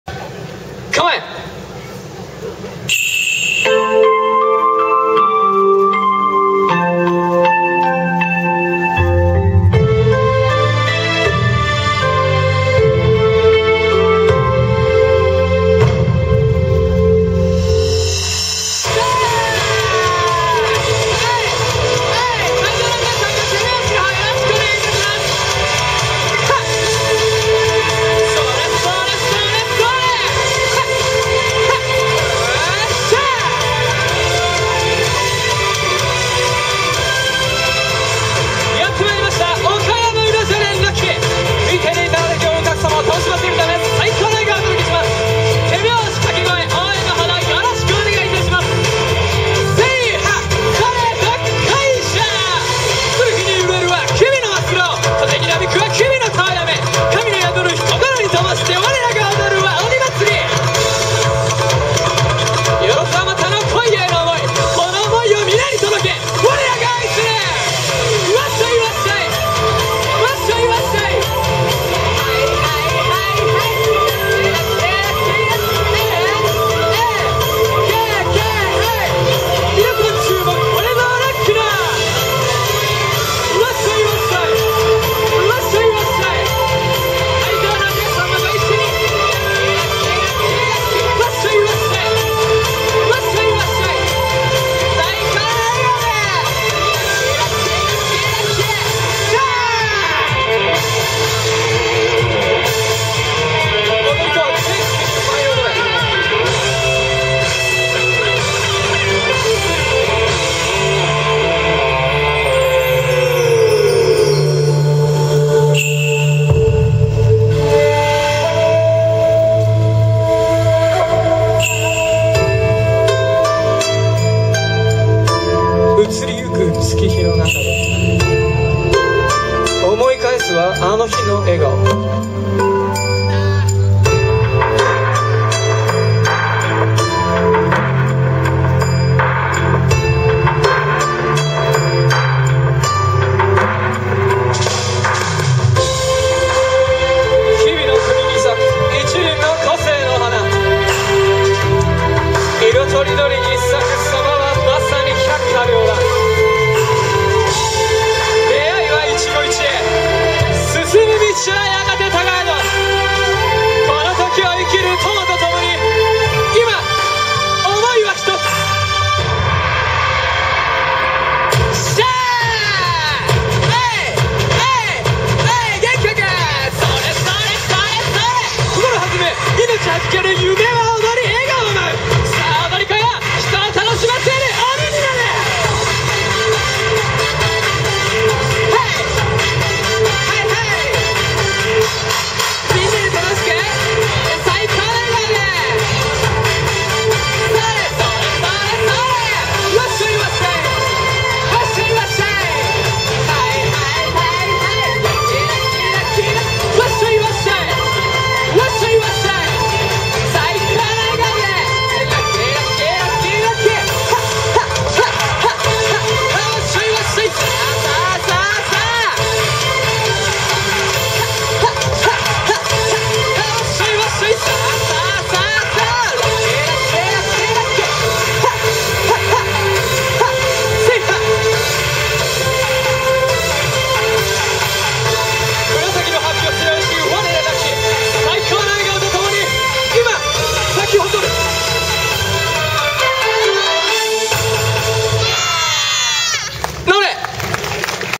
楽鬼 2010 こいや祭り １日目 メイン会場
2010年９月１１日に万博ファームエキスポメイン会場でおこなわれた　第１１回こいや祭り　１日目の　楽鬼さんの　ステージ演舞です